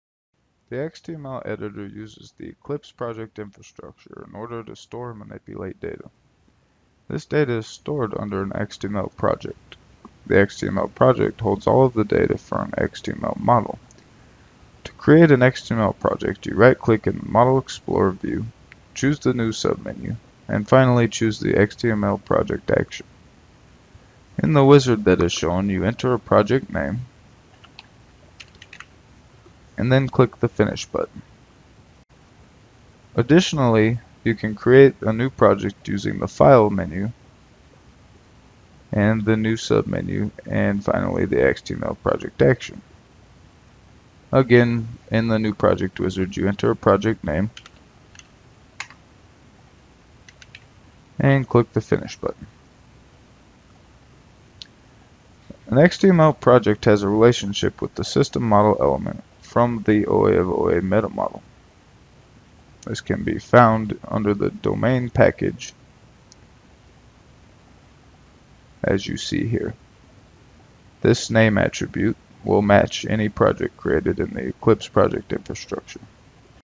xtUML Projects
This video describes how to create xtUML Projects and describes how projects are tied into the xtUML meta-model.